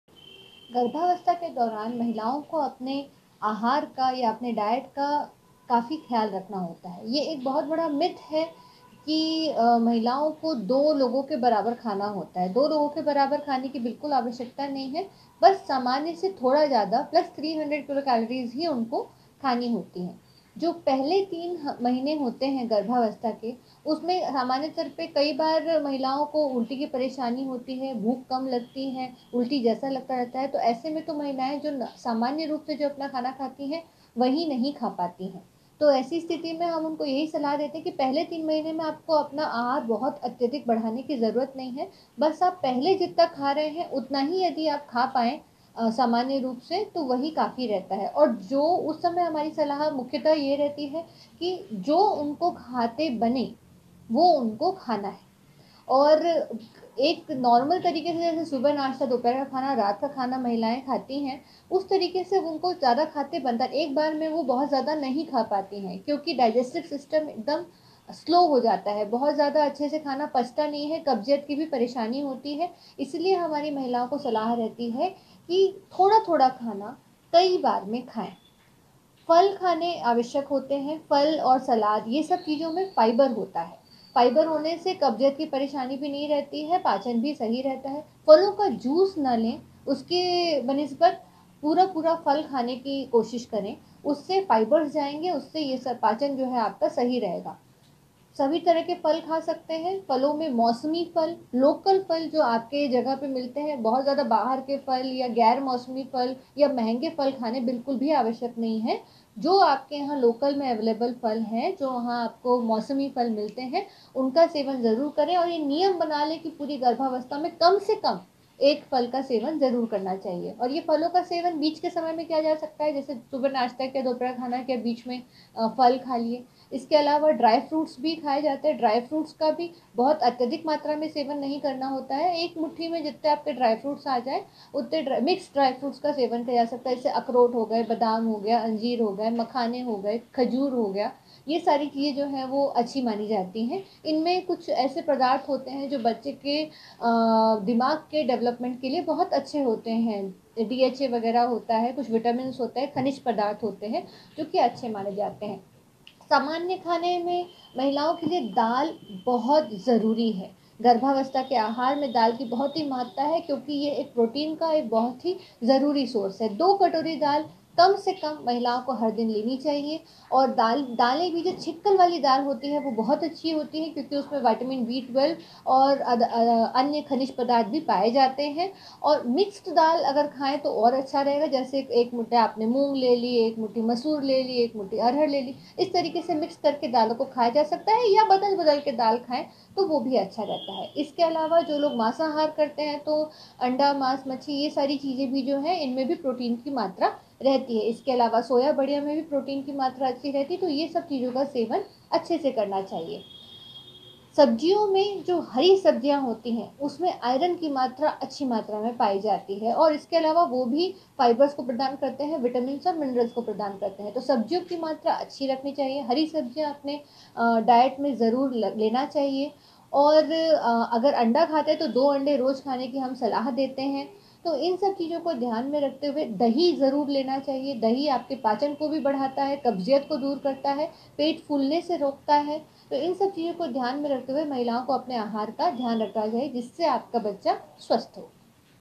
Healthy Diet During Pregnancy : प्रेग्नेंसी में रखिए प्रोटीन, विटामिन और कैल्शियम का ध्यान
Pregnancy Healthy Diet Plan : प्रेग्नेंसी के दौरान महिलाओं को अपने डाइट का खास ख्याल रखना चाहिए. हेल्दी डाइट का बच्चे के शरीरिक और मानसिक विकास पर पूरा असर होता है.

You Can follow ETV Bharat Chhattisgarh ( ईटीवी भारत छत्तीसगढ़ ) On:-